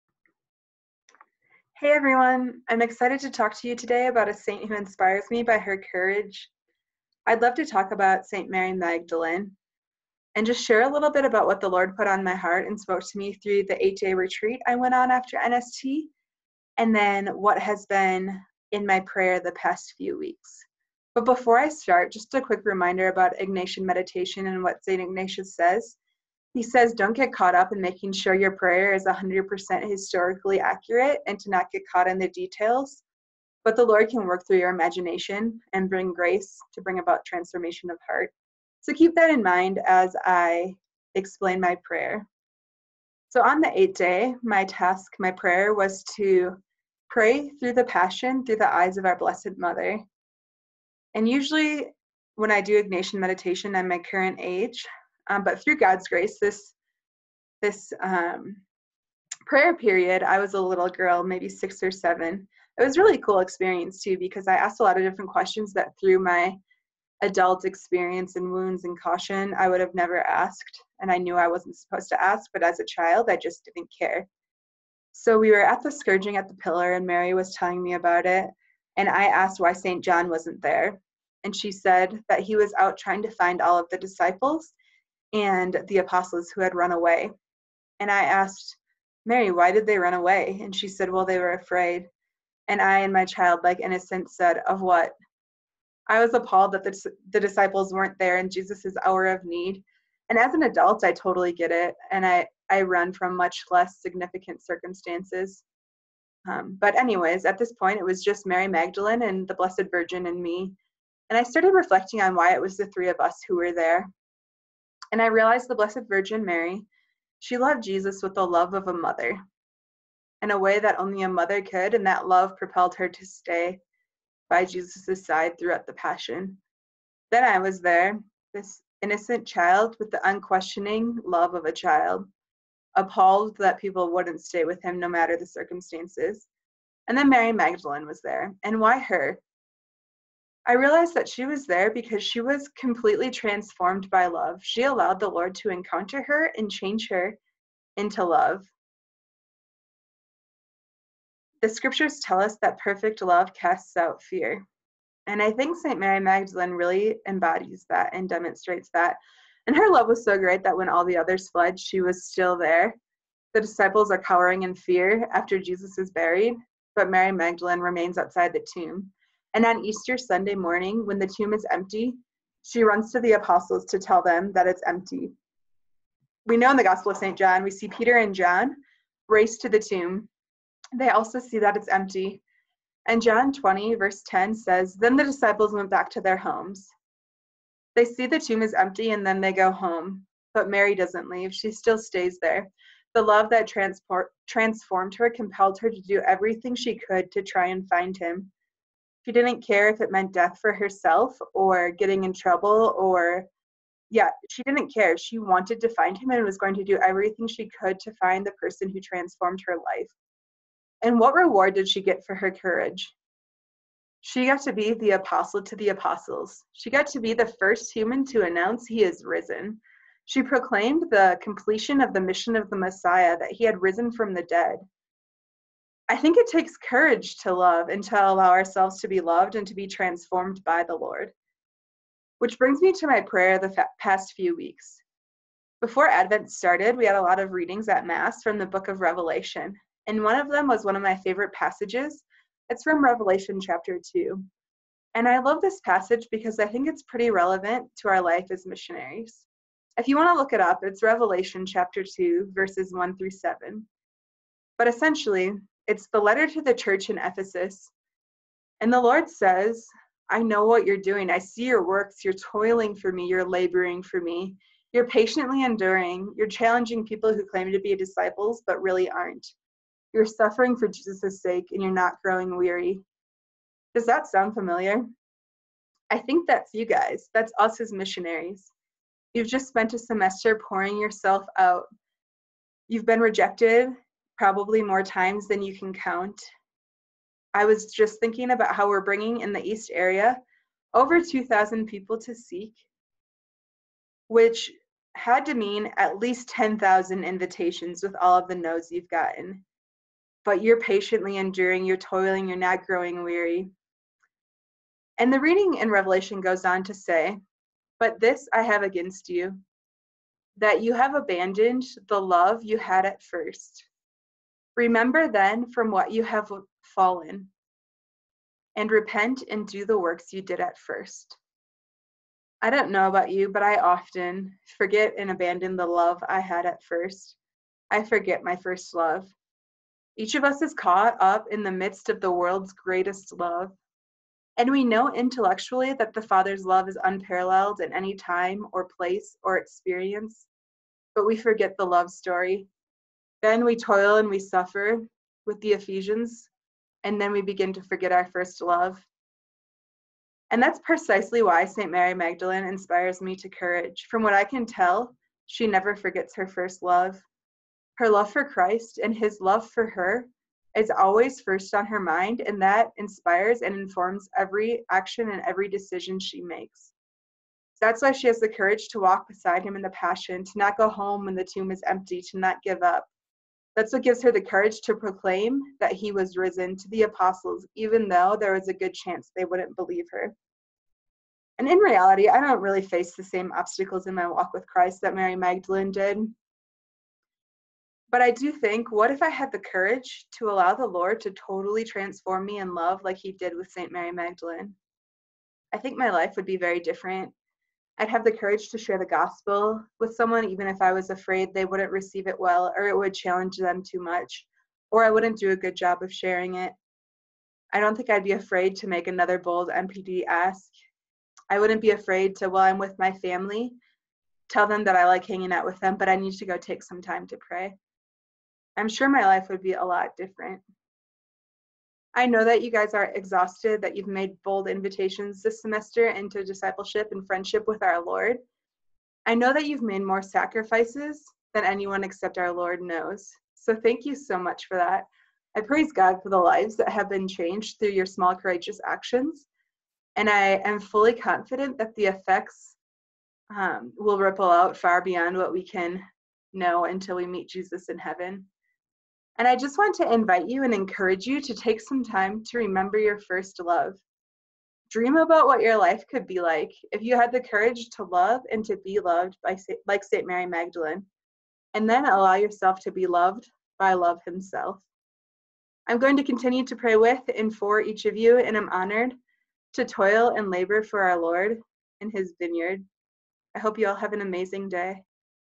St Mary Magdalene
A brief Ignatius meditation the love of St. Mary Magdalene.